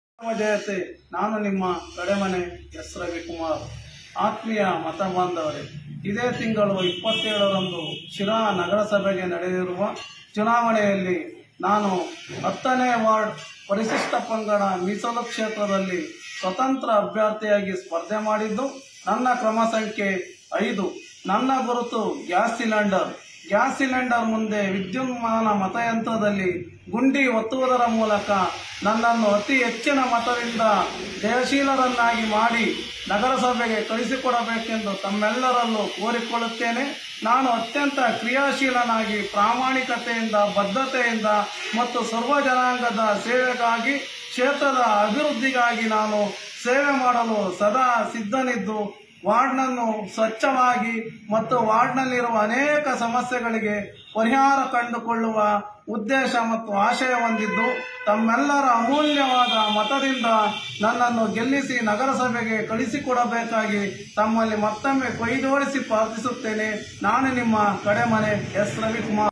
Sri Tv Sira 24-12-2021
ಅಬ್ಬರದ ಪ್ರಚಾರ.
ದಿನಾಂಕ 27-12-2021ನೇ ಸೋಮವಾರ ನಡೆಯಲಿರುವ ಶಿರಾ ನಗರ ಸಭೆ ಚುನಾವಣೆಯಲ್ಲಿ 10ನೇ ವಾರ್ಡಿನ ಪಕ್ಷೇತರ ಅಭ್ಯರ್ಥಿಯಾಗಿ ಸ್ಪರ್ಧಿಸಿರುವ ಕಡೆಮನೆ ರವಿಕುಮಾರ್ ಆದ ನನಗೆ ನಿಮ್ಮ ಅಮೂಲ್ಯವಾದ ಮತವನ್ನು ಗ್ಯಾಸ್ ಸಿಲಿಂಡರ್ ಗುರುತಿನ ಮುಂದೆ ಎಲೆಕ್ಟ್ರಾನಿಕ್ ಬಟನ್ ಒತ್ತುವುದರ ಮೂಲಕ ನನ್ನನ್ನು ಜಯಶೀಲನನ್ನಾಗಿ ಮಾಡಬೇಕೆಂದು ತಮ್ಮಲ್ಲಿ ಕಳಕಳಿ ಮನವಿ. 🙏🙏🙏